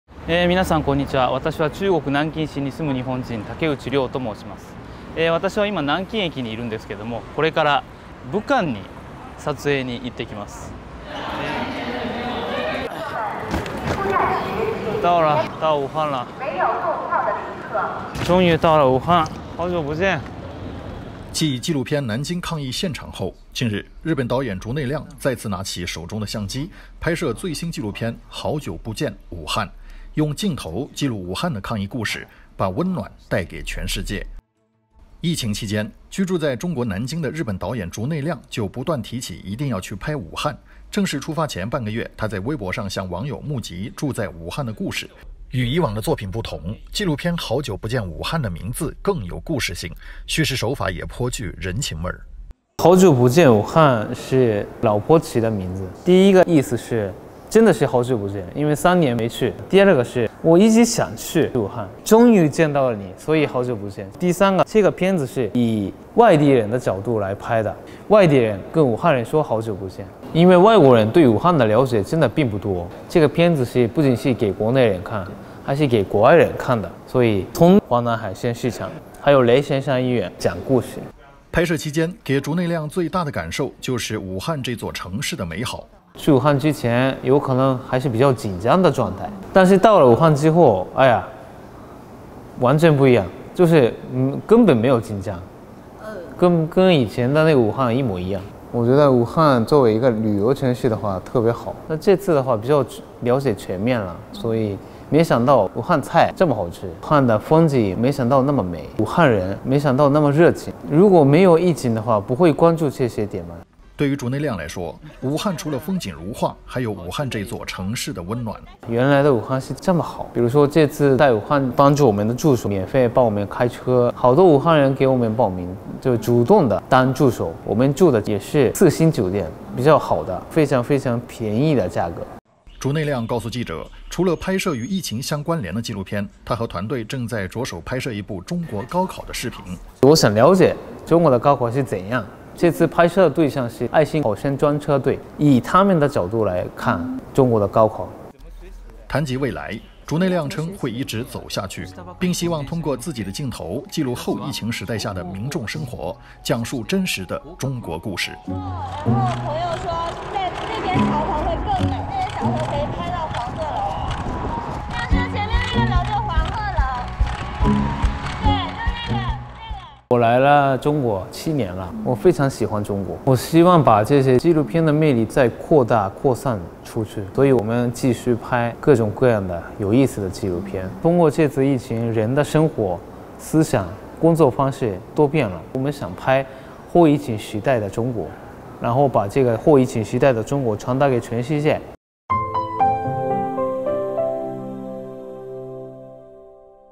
日本导演竹内亮：希望记录“后疫情时代”的中国
继纪录片《南京抗疫现场》后，近日，日本导演竹内亮再次拿起手中的相机，拍摄最新纪录片《好久不见，武汉》，用镜头记录武汉的抗疫故事，把“温暖”带给全世界。拍摄期间，给竹内亮最大的感受就是武汉这座城市的“美好”。